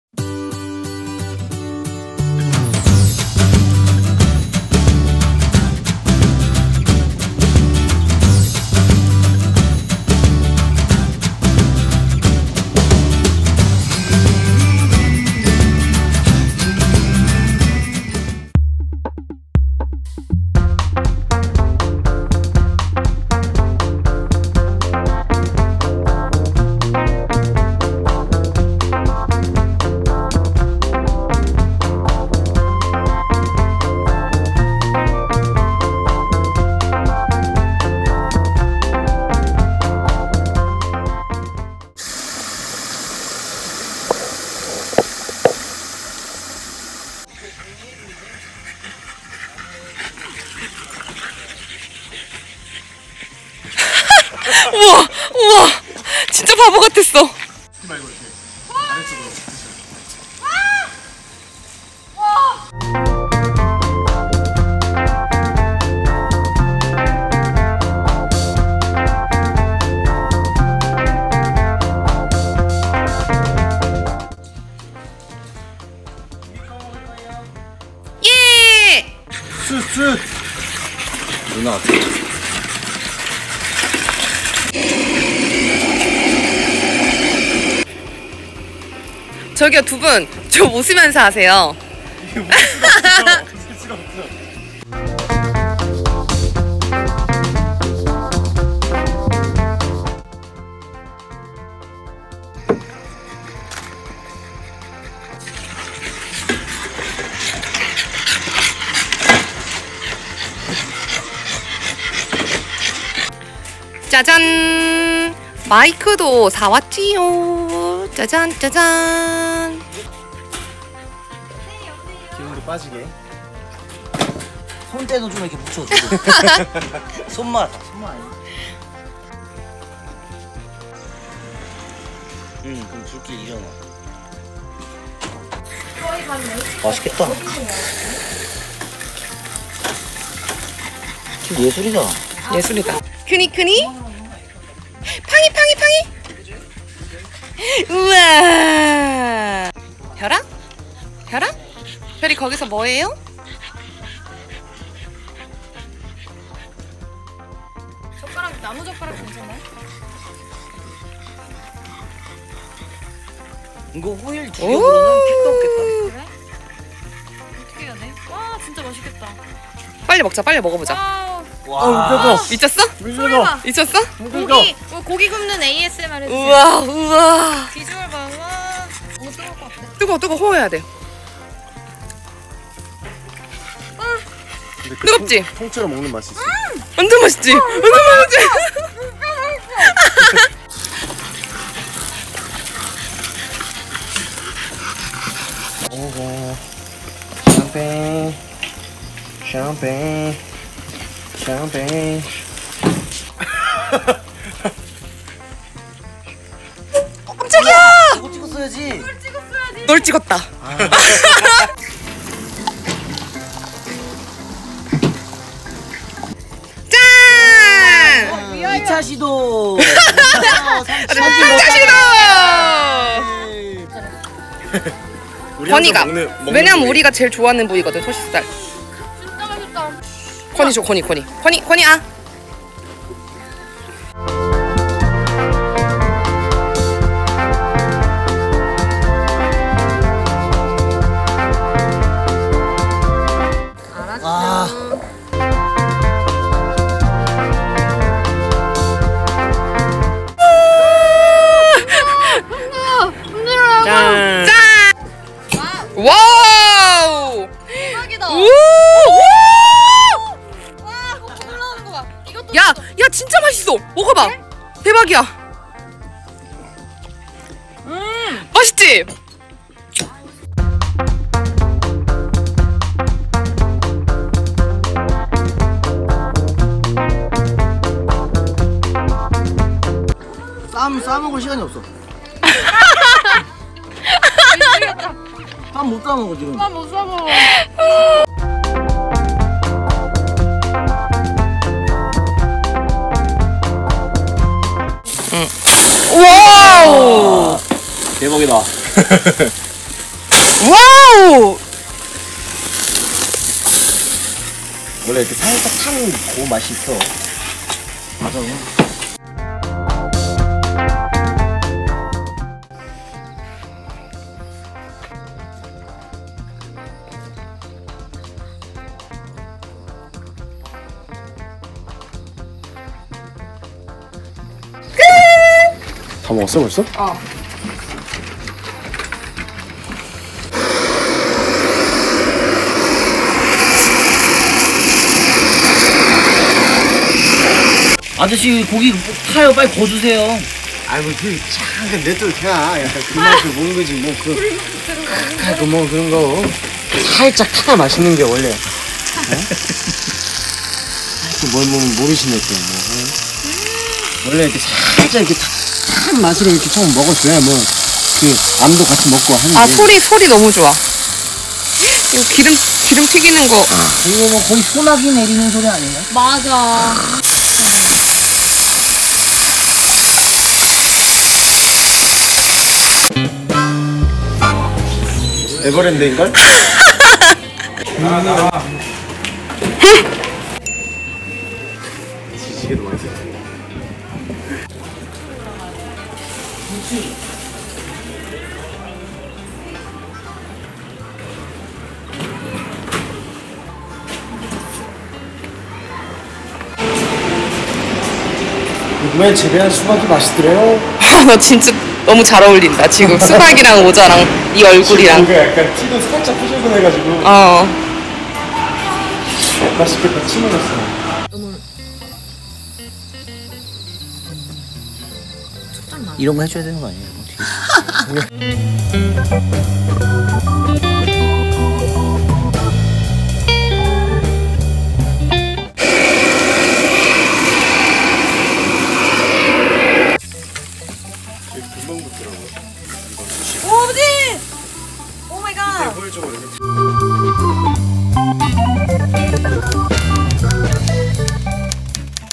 [권댕House]Vlog 역대급 비주얼 바베큐파티/소고기+돼지고기+양고기+맥주
애기들 물놀이 영상에 이어 이번에 바베큐 파티 브이로그여유
너무 맛있었어서 영상 편집하면서 또 고기가 먹고 싶어졌어유.
늦은 시간에 올려서 죄송해유
다들 참지말고 드셔유
다이어트는 원래 내일부터예유~